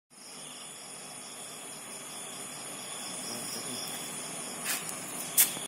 Wood Design Paint For Gate Grill
Final touch up before delivery the gate...